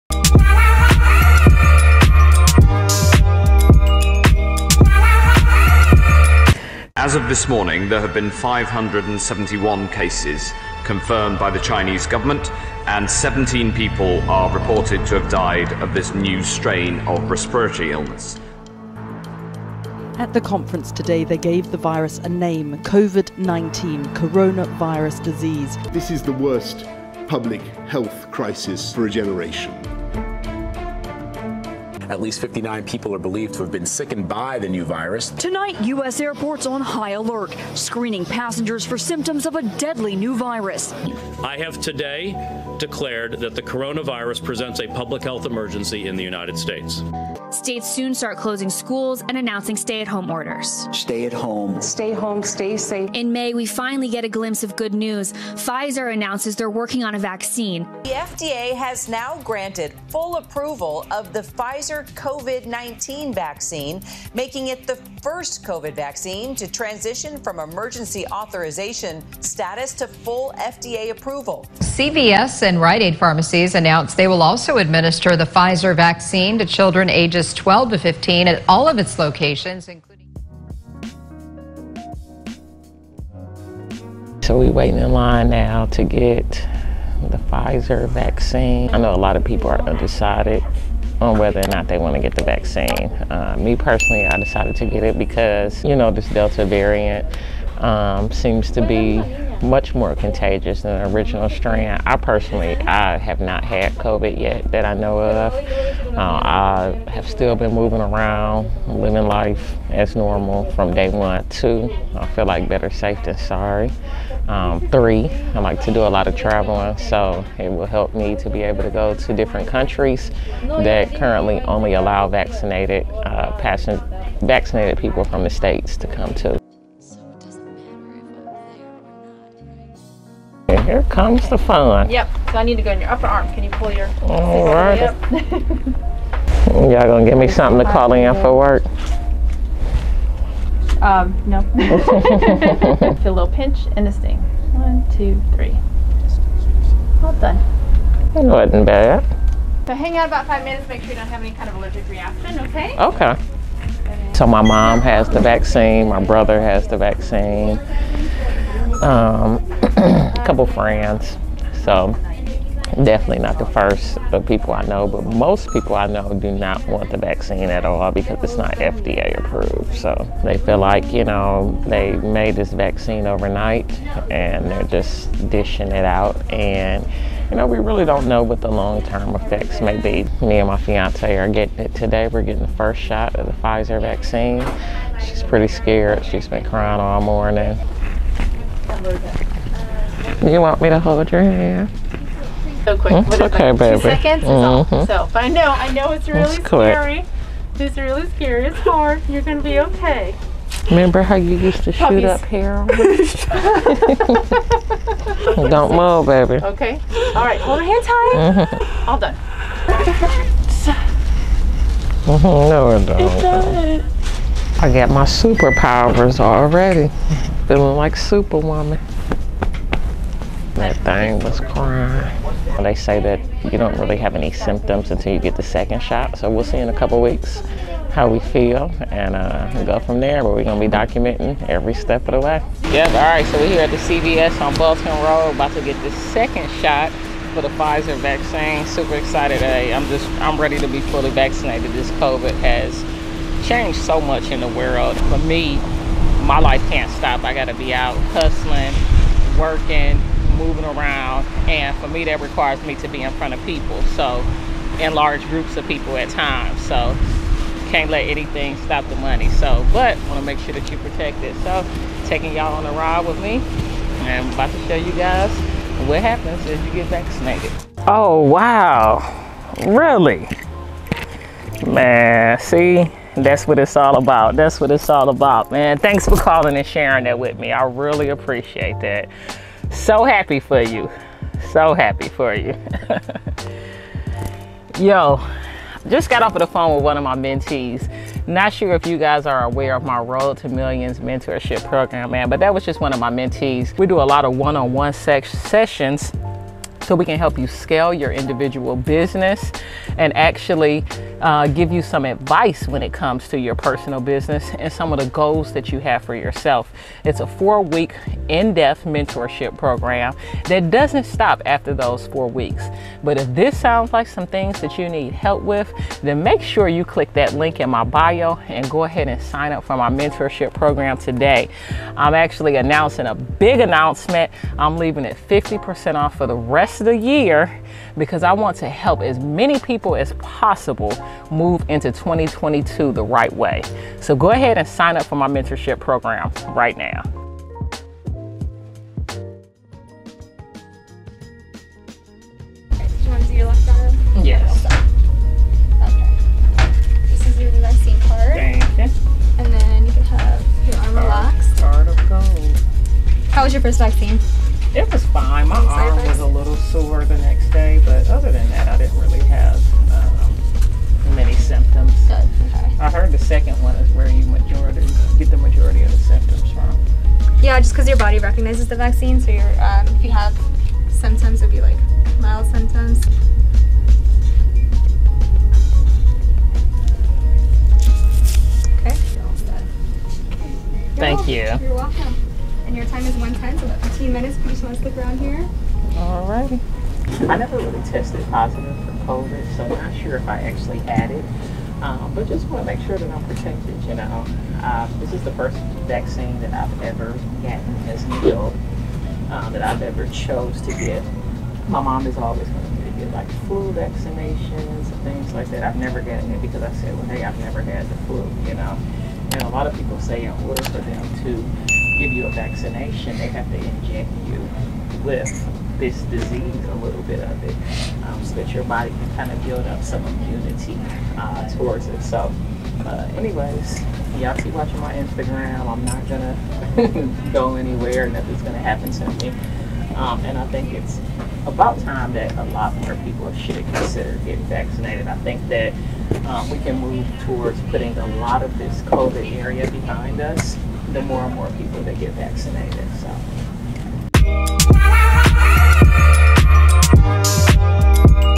Pfizer COVID 19 Vaccine Demo - WhoIsSnoop
Here's an quick guide explaining the Pfizer vaccination full process. Pfizer-BioNTech (COMIRNATY) received U.S. Food and Drug Administration (FDA) approval on August 23, 2021, for individuals 16 years of age and older.